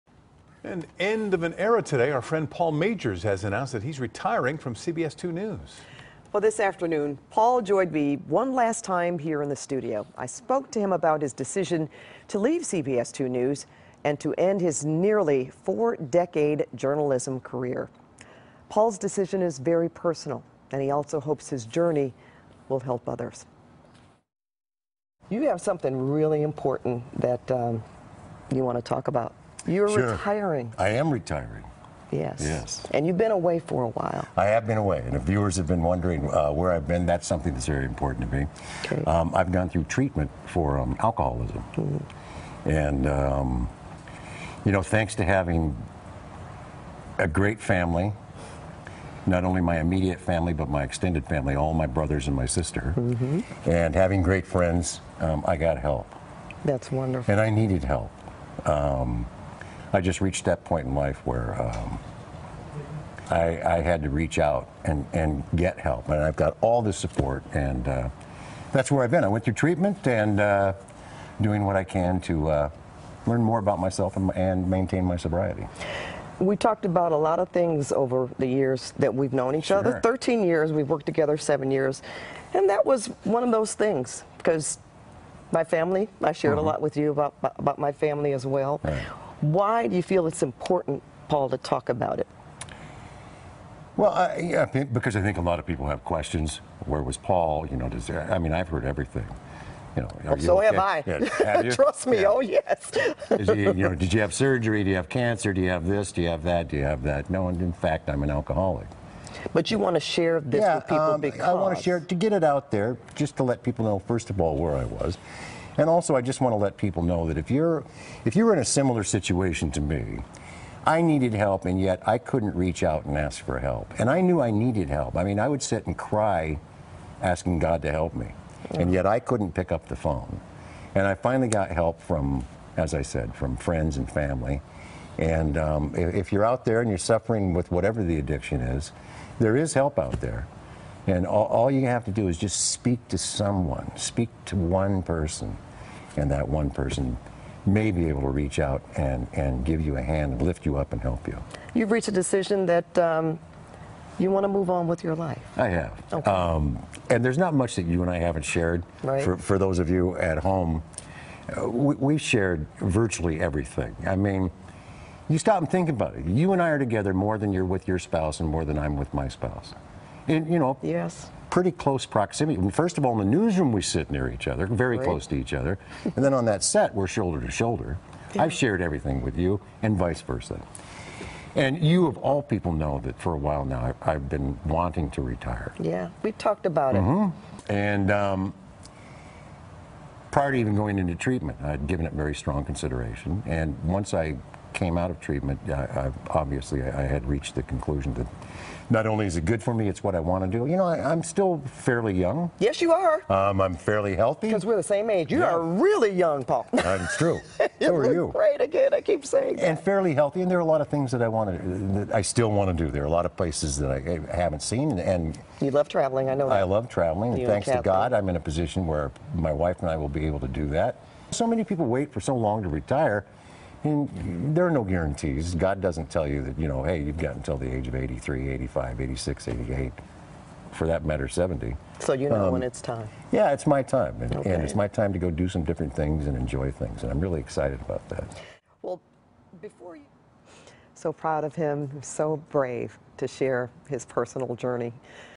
Paul Magers Announces Retirement From CBS2 News
Longtime anchorman says he has been in treatment for addiction to alcohol. Pat Harvey reports.